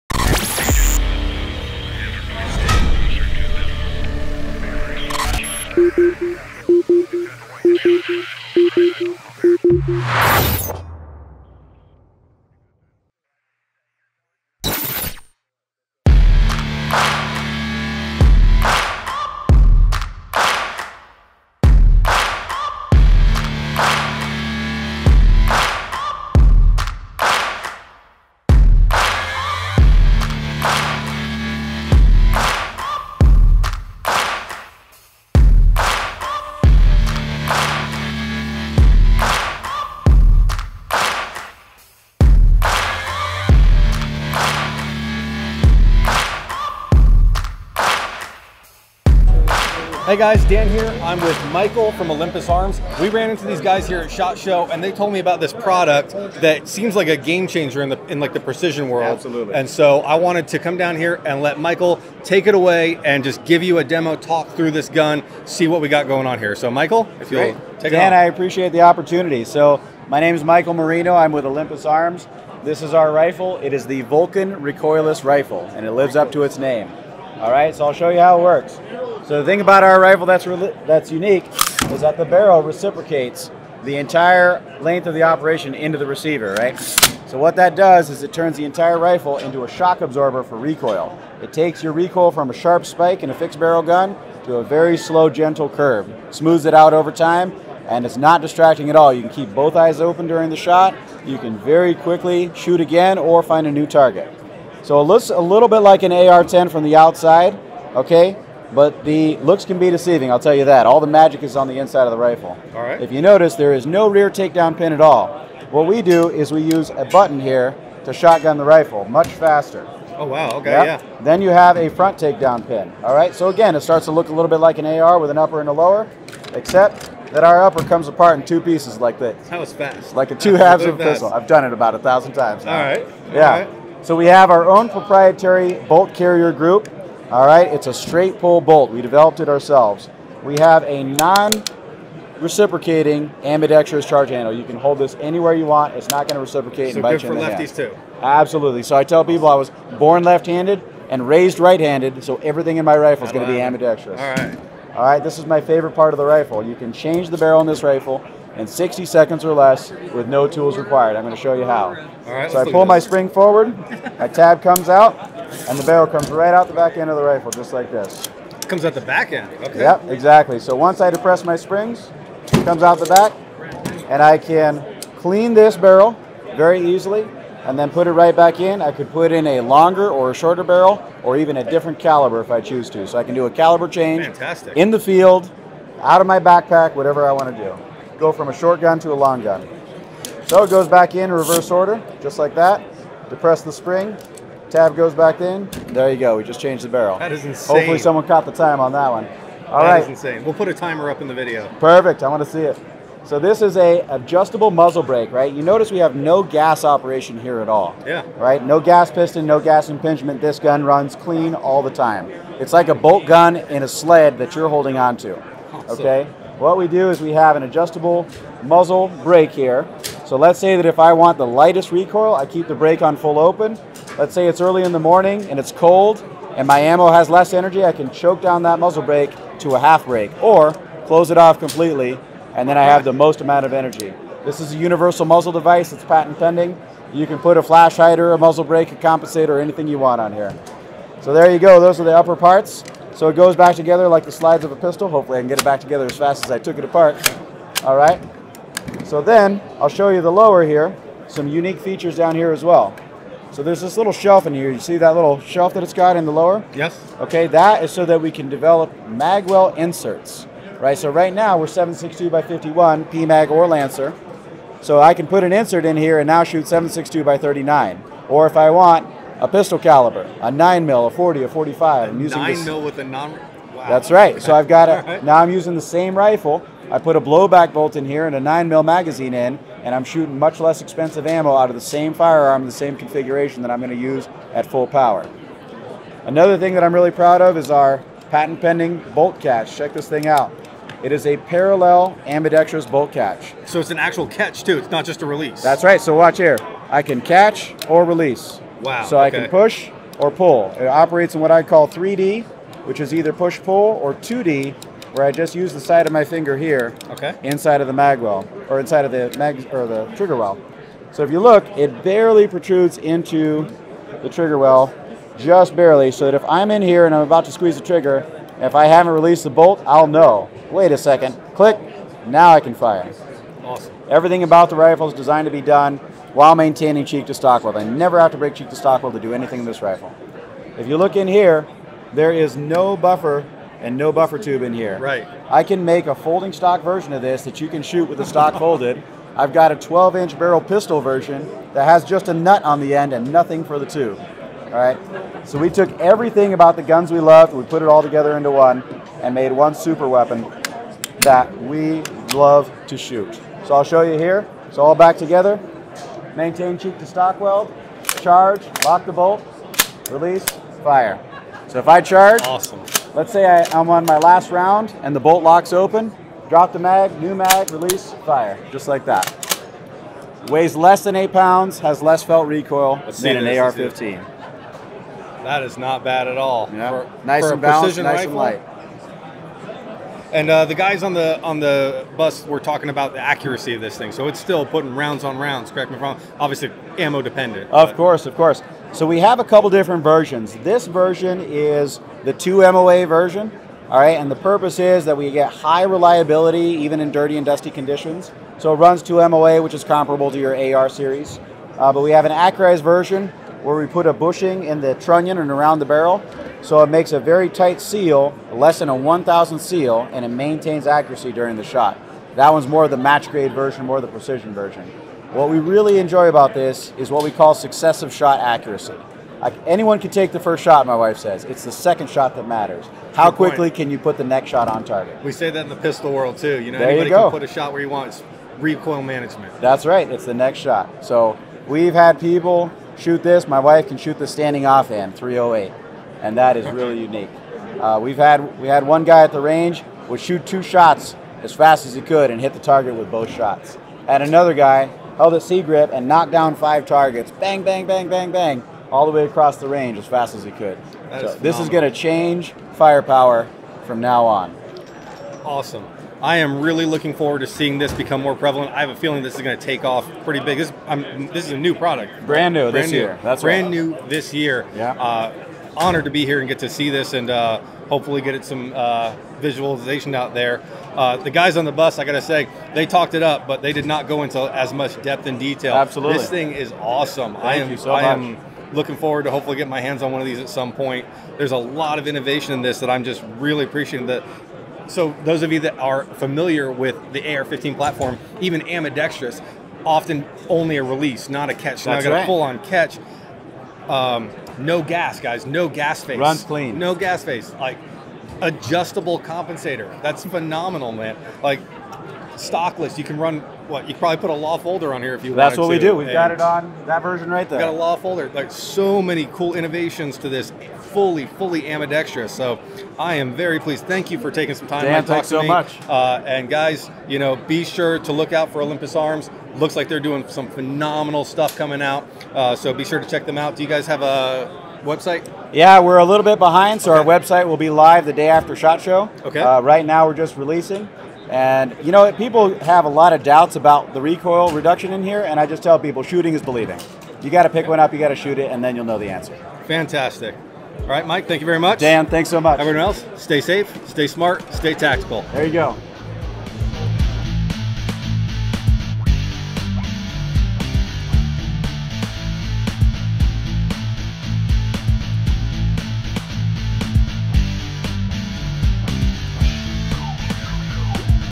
Olympus Arms Vulcan!- True Innovation at SHOT Show 2023
Olympus Arms unveiled the new Vulcan Recoiless Rifle.  In this video, Michael Merino goes over the new technologies and improvements that have been added into what could be misidentified as an AR10, but is in fact creating a whole new class of carbine/rifle itself.

SPG1 Tactical
S2S Gun Lube
"The 6 Bullets You Can't Leave Home Without" book